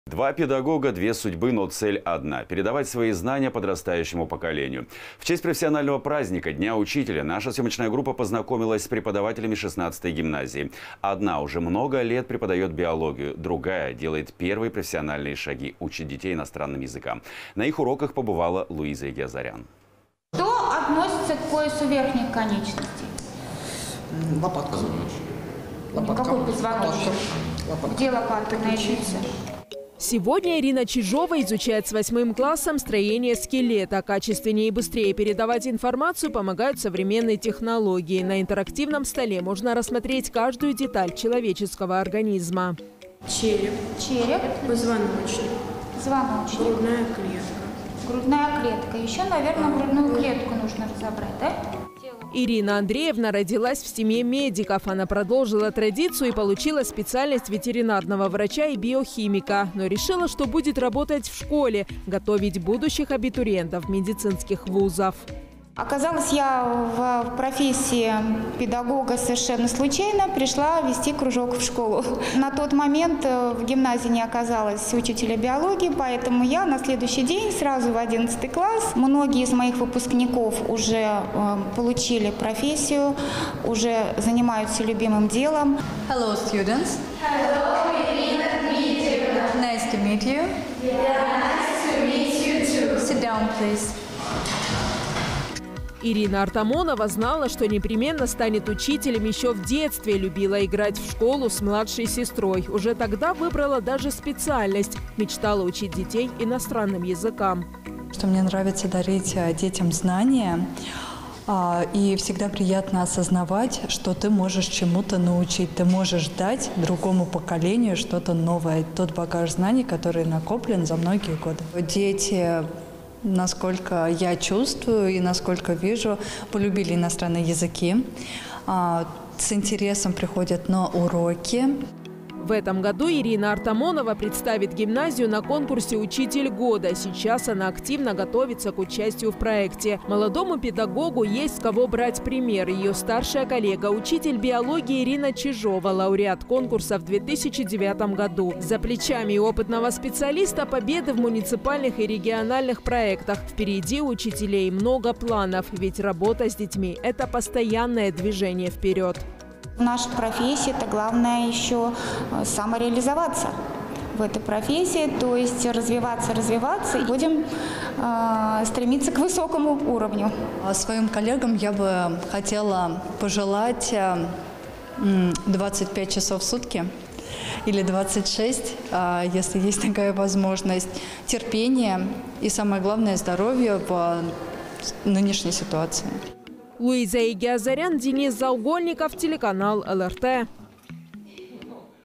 Два учителя - две судьбы
Два педагога, две судьбы, но цель одна – передавать свои знания подрастающему поколению. В честь профессионального праздника – Дня учителя – наша съемочная группа познакомилась с преподавателями 16 –ой гимназии. Одна уже много лет преподает биологию, другая – делает первые профессиональные шаги, учит детей иностранным языкам. На их уроках побывала Луиза Егиазарян.

Сегодня Ирина Чижова изучает с 8 классом строение скелета. Качественнее и быстрее передавать информацию помогают современные технологии. На интерактивном столе можно рассмотреть каждую деталь человеческого организма.

Ирина Андреевна родилась в семье медиков. Она продолжила традицию и получила специальность ветеринарного врача и биохимика. Но решила, что будет работать в школе – готовить будущих абитуриентов медицинских ВУЗов.

Ирина Чижова, учитель биологии гимназии №16 г.о. Люберцы:
«Оказалась я в профессии педагога совершенно случайно, пришла вести кружок в школу. На тот момент в гимназии не оказалось учителя биологии, поэтому я на следующий день сразу в 11-ый класс и вот эта стихия меня понесла, понесла, и вот уже 19 лет. Самое главное, в моей профессии это когда я вижу своих выпускников успешными, состоявшимися. Многие из моих выпускников уже получили профессию, уже занимаются любимым делом».

Ирина Артамонова знала, что непременно станет учителем еще в детстве. Любила играть в школу с младшей сестрой. Уже тогда выбрала даже специальность – мечтала учить детей иностранным языкам.

Ирина Артамонова, учитель английского и немецкого языков гимназии №16 г.о. Люберцы:
«Мне нравится дарить детям знания. Всегда приятно осознавать, что ты можешь чему-то научить, ты можешь дать другому поколению что-то новое, тот багаж знаний, который накоплен за многие годы. Дети, насколько я чувствую, и насколько я вижу, полюбили иностранные языки, с интересом приходят на уроки».

В этом году Ирина Артамонова представит гимназию на конкурсе «Учитель года». Сейчас она активно готовится к участию в проекте. Молодому педагогу есть, с кого брать пример. Её старшая коллега - учитель биологии Ирина Чижова – лауреат конкурса в 2009 году. За плечами у опытного специалиста победы в муниципальных и региональных проектах. Впереди у учителей - много планов. Ведь работа с детьми – это постоянное движение вперед.

«В нашей профессии главное – самореализоваться, то есть, развиваться, развиваться. Будем стремиться к высокому уровню».

«Своим коллегам я бы хотела пожелать 25 часов в сутки или 26, если есть такая возможность, терпения и, самое главное, здоровья в нынешней ситуации».